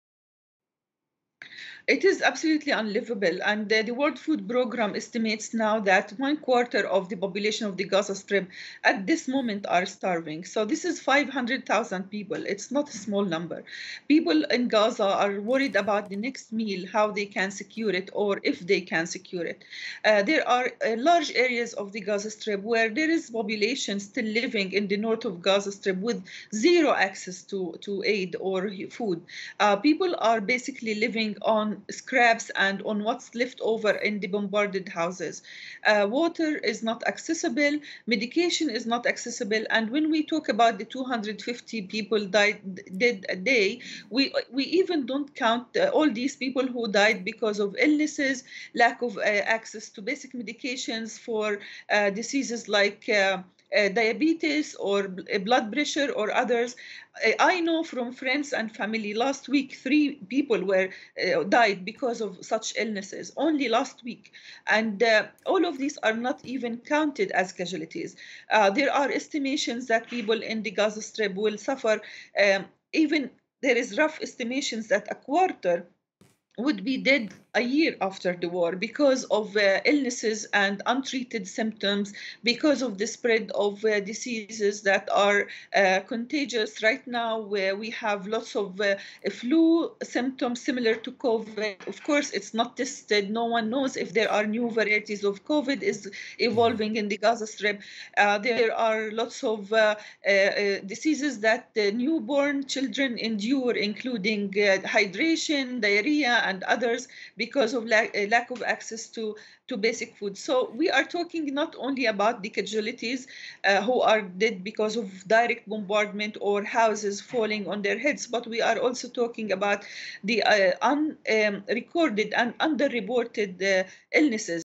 Gaza Strip 'absolutely unliveable', says Oxfam spokesperson • FRANCE 24 English
Most of Gaza's 2.3 million people have been forced from their homes in a strip that has been in large part laid waste, where the health system has almost collapsed, and the United Nations says restrictions on food and medical aid deliveries have left thousands hungry and at risk of disease. “It is absolutely unliveable,” Dr. Hadeel Qazzaz, Oxfam international spokesperson in the occupied West Bank, told FRANCE 24. “When we talk about 250 people killed a day, we don’t count people who die of illnesses or lack of access to medications.”